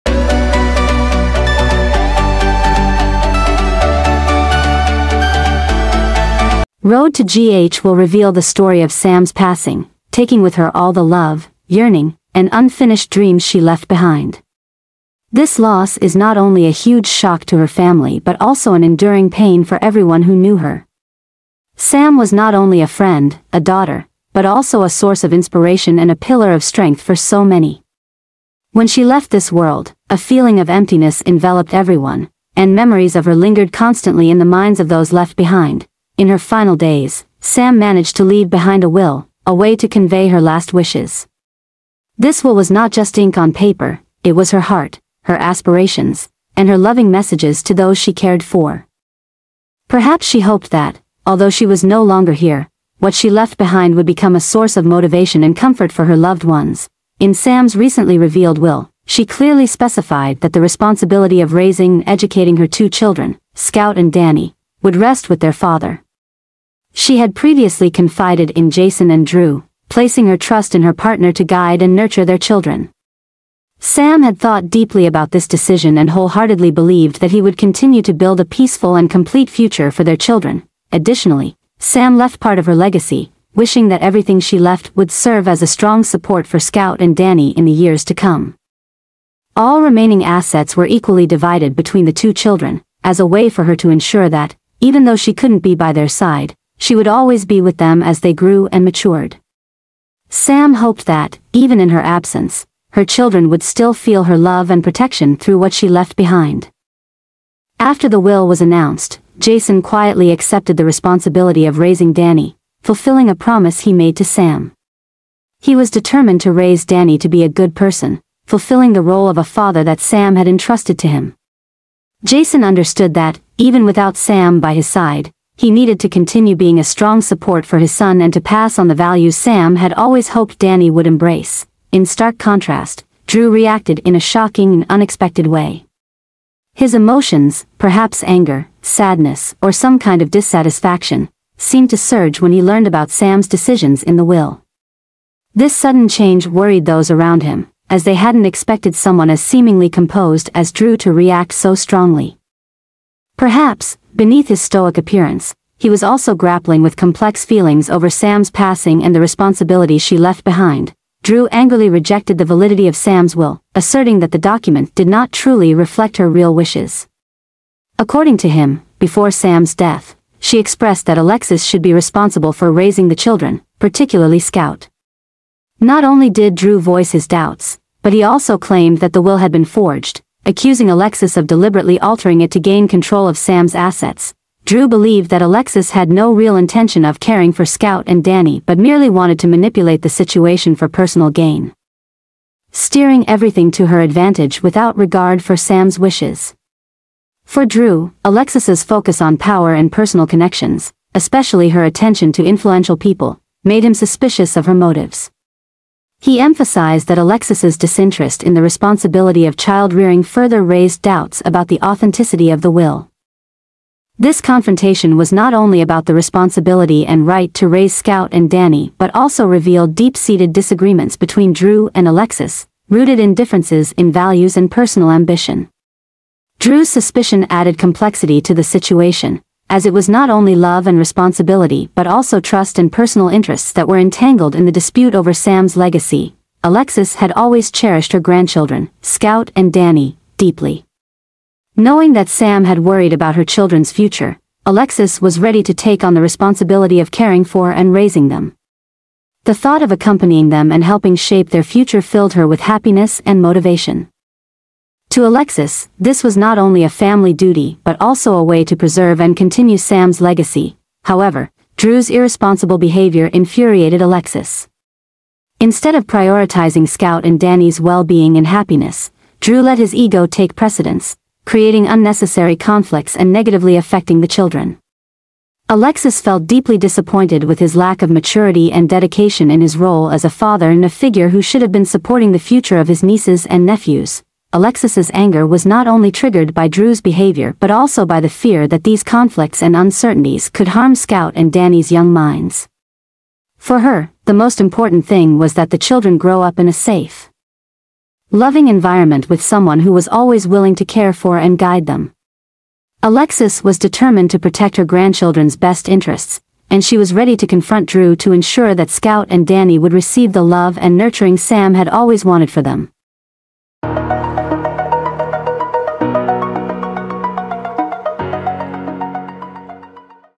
Drew accuses Alexis of forging Sam's will to avoid raising Scout | GH Spoilers
Drew accuses Alexis of forging Sam's will to avoid raising Scout